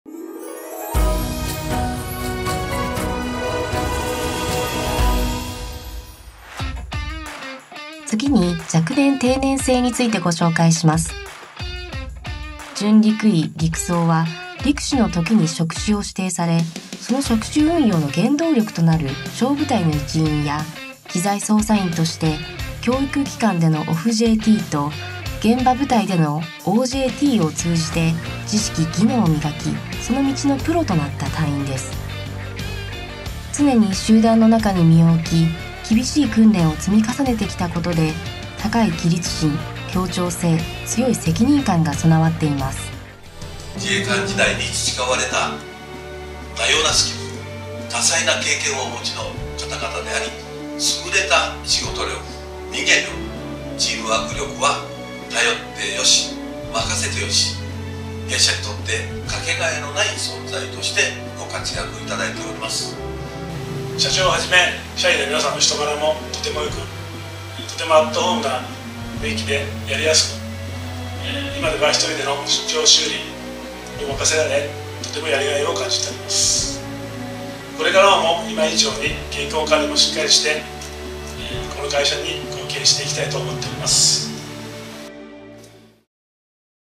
陸上自衛隊再就職支援動画 准曹自衛官
自衛隊では組織の精強性を維持する必要があることから、「再就職による第２の人生を前提」として、一般的な定年年齢より若くして退職する制度を採り入れています。
如何なる任務も確実に達成できるように、教育・訓練を通じて、様々な能力を培った約８０００名の人材が、毎年全国各地でその任務を終えて退職を迎えています。
退職自衛官の採用にご関心ある方はコチラもチェックして下さい！
更にそれぞれの項目について詳しい内容が知りたい方はコチラの動画もご確認ください！